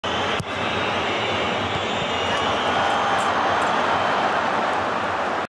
Tooor! Eckball im Amiri-Style🤩
Einfach mal direkt reingeschweißt die Ecke ⚽️ 💥